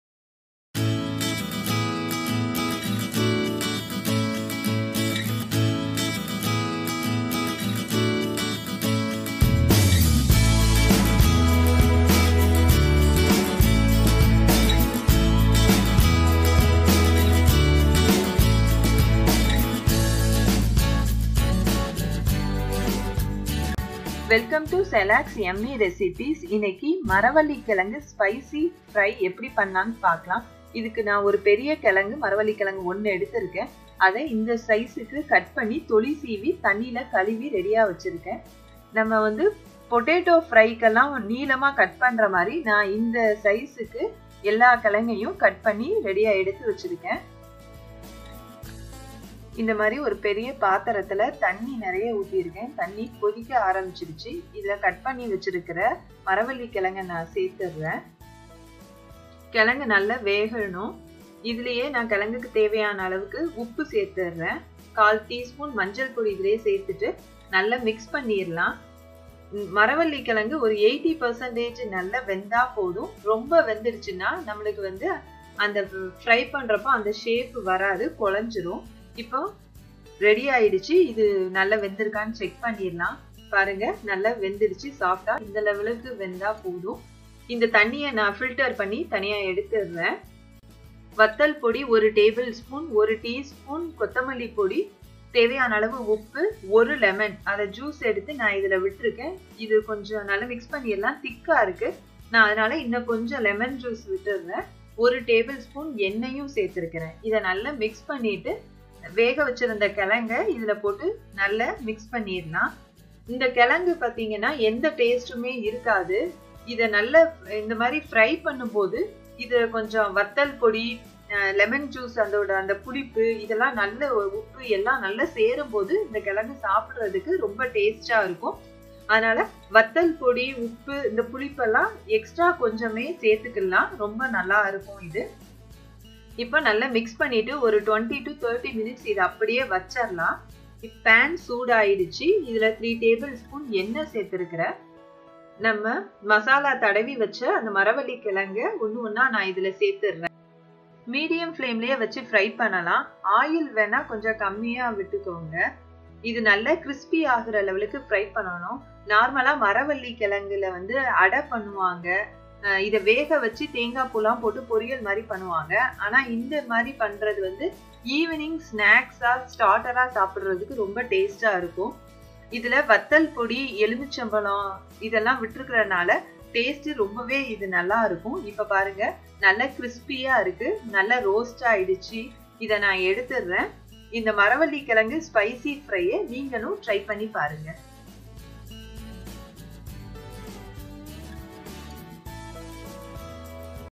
மரவள்ளிக்கிழங்கில் இப்படி செய்து பாருங்க /Maravalli Kilangu Fry /Maravalli kilangu Recipes in Tamil
In this video we will see how to make Tapioca Fry |  மரவள்ளிக்கிழங்கு  வறுவல் | Maravalli Kizhangu Fry | Kerala Kappa Fry  in Tamil || How to make Tapioca Fry in tamil / Maravalli Kizhangu Fry

Ingredients
Tapioca / Maravalli
Kizhangu – 1 medium size
Lemon - 2
Salt – as per taste
Oil - 4tbsp

Happy Cooking!